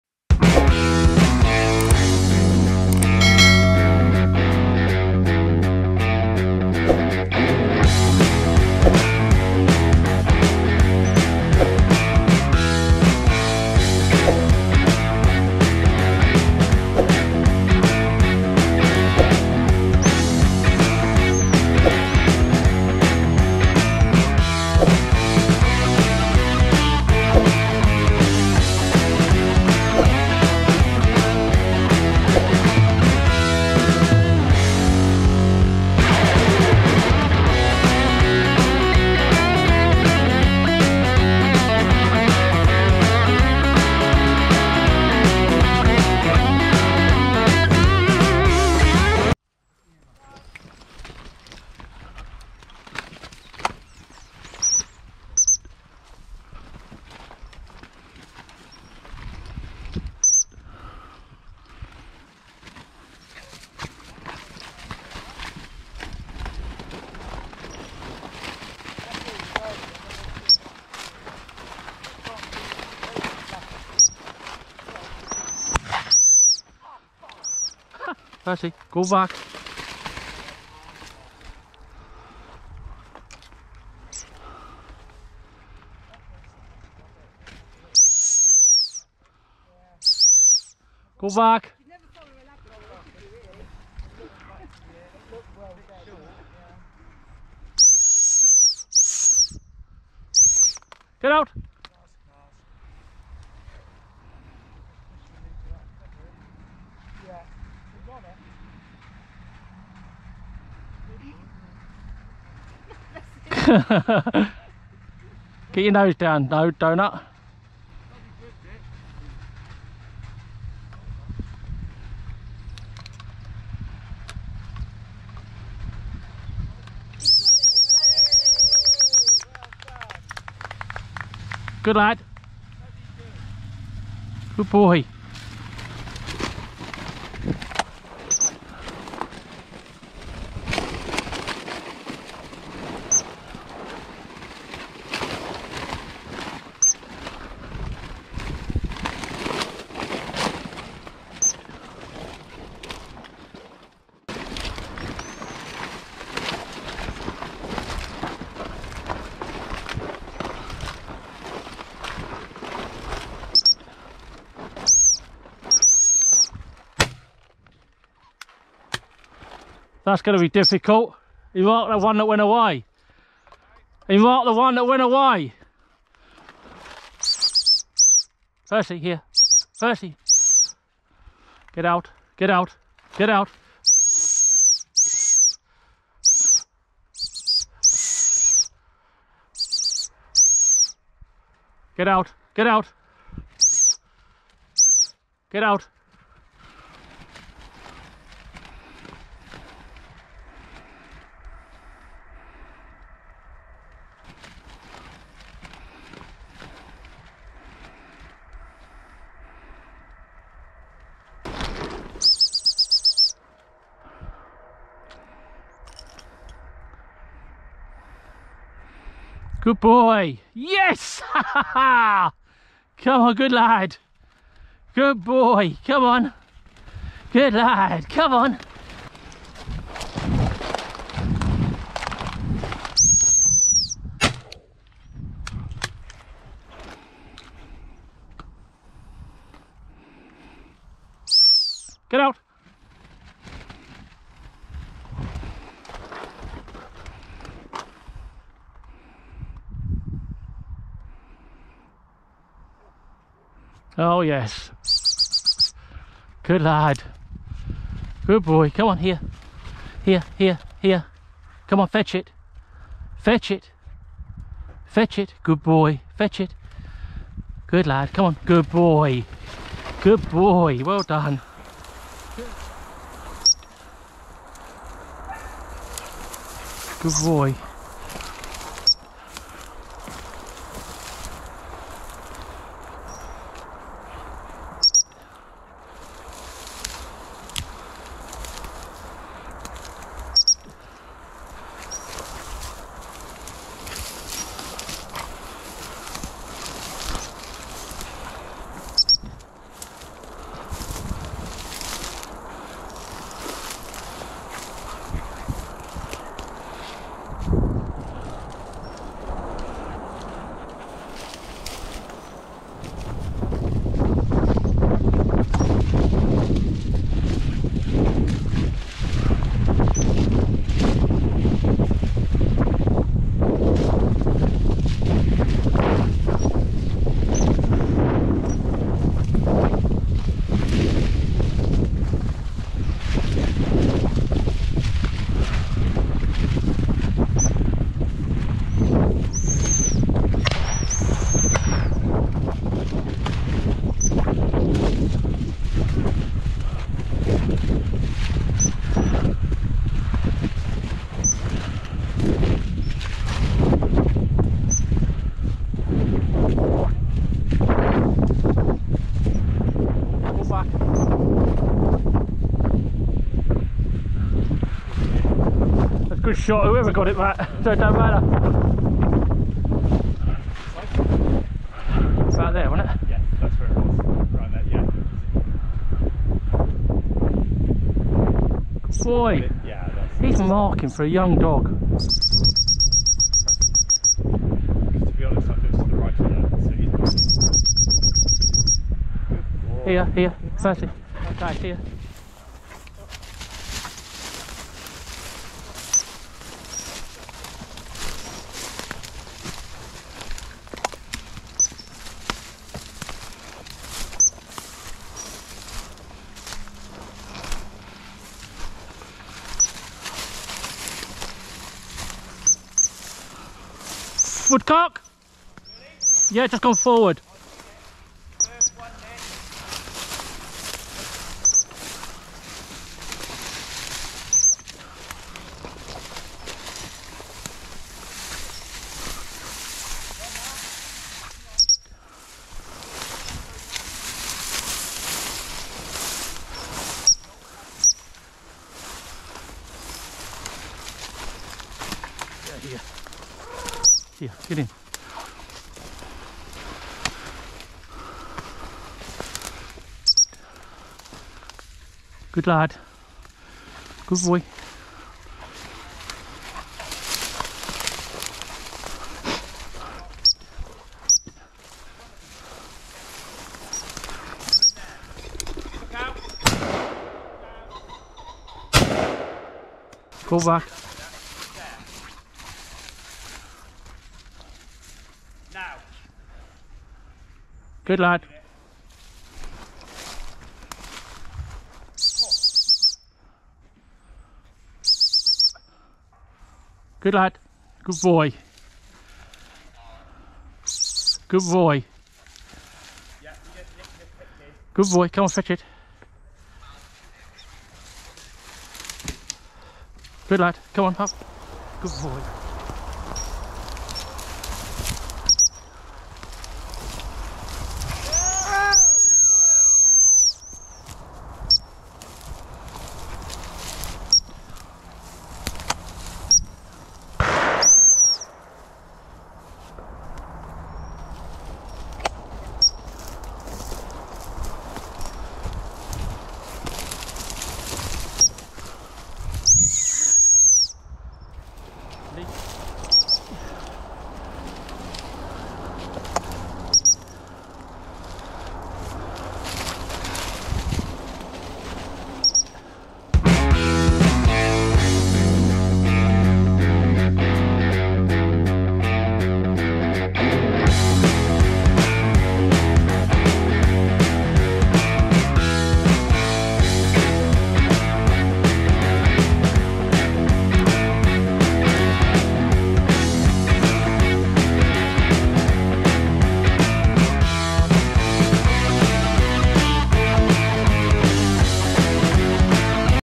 Gundog Training - Percy’s Progress - So Far So Good!
Over the past couple of months I have been gradually introducing Percy to the shooting field. Although there are a few issues that will need to be addressed he is generally doing very well, when he is switched on his hunting is really very nice, his marking is very good for such a young dog and his retrieving is looking quite tidy.  He is still very much “a work in progress” but we are making progress and heading in the right direction…..